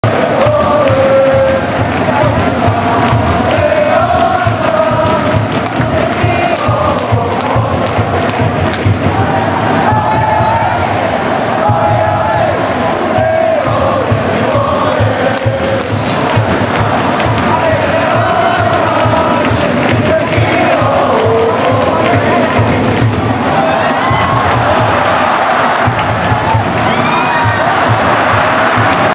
3点リードされても逆転優勝へ「奇跡を起こせ」の声援！（Jリーグ2023第30節 ヴィッセル神戸VS鹿島アントラーズ）
2023年のJリーグJ1第30節で、ヴィッセル神戸がホームゲームとして国立競技場で開催し、2020年元旦の天皇杯決勝のカードとなる、ヴィッセル神戸VS鹿島アントラーズの対戦となりました。
後半の残り時間が無くなっても、鹿島アントラーズのサポーターからは逆転優勝へかすかな望みをかけて「奇跡を起こせ」の声援を贈っています。